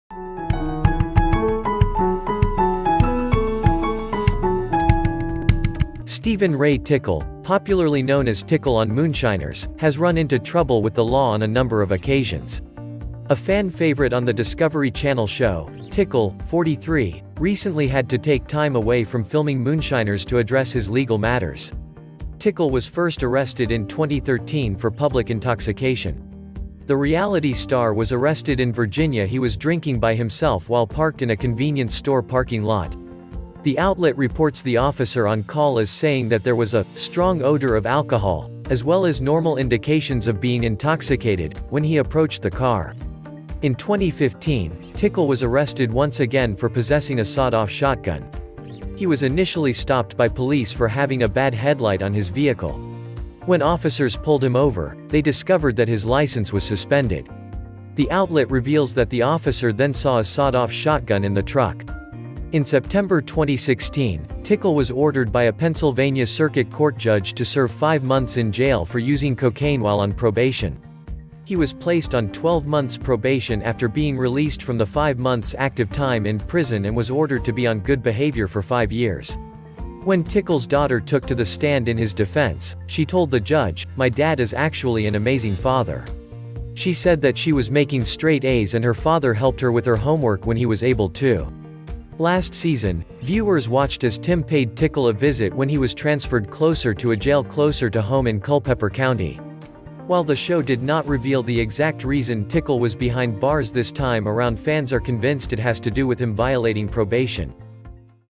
Why was Tickle from Moonshiners in Jail? Where is he now?
Steven Ray Tickle, popularly known as Tickle on Moonshiners, has run into trouble with the law on a number of occasions. Find out why he was sent to jail and his encounters with the law.
We are very grateful that you are watching our videos. We upload new videos everyday.